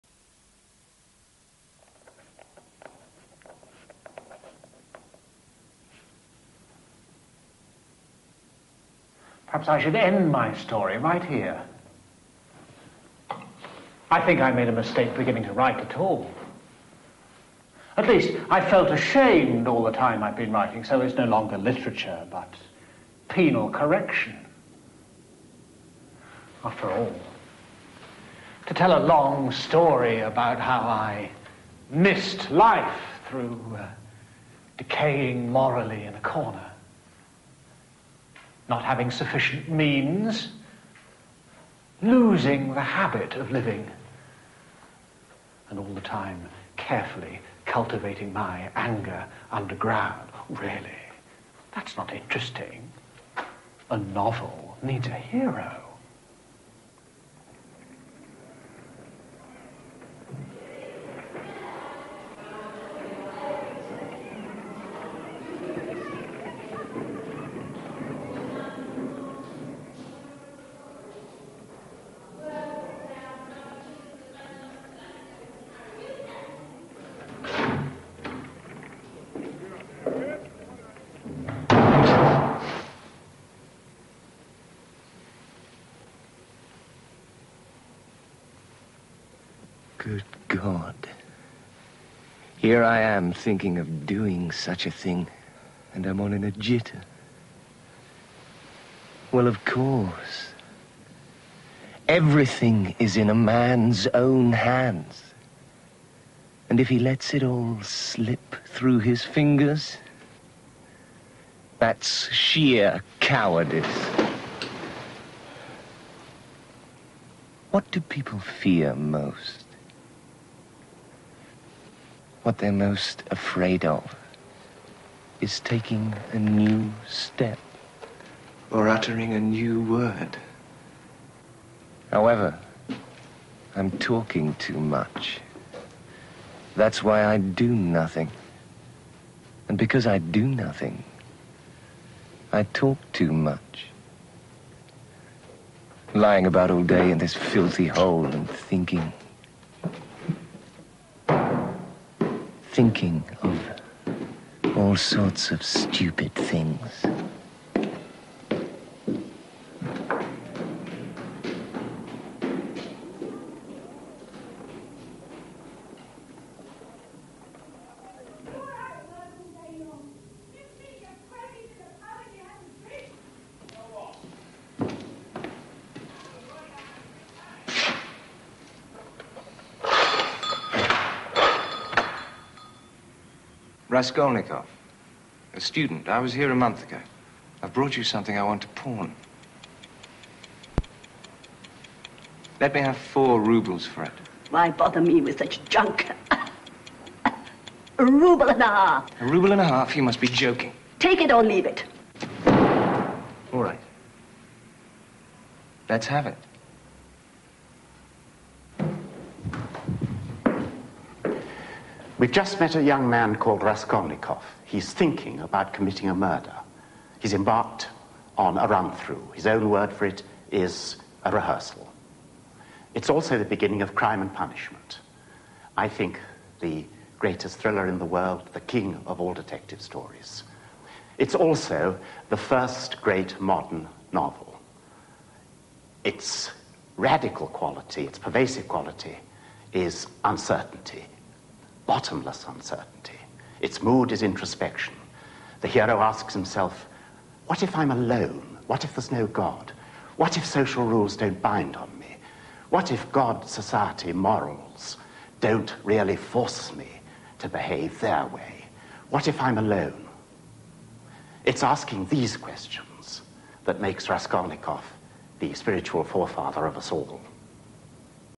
Crime & Punishment Documentary Part 1 of 8
Documentary on Dostoevsky and his famous work "Crime and Punishment". Also features "Notes From Underground".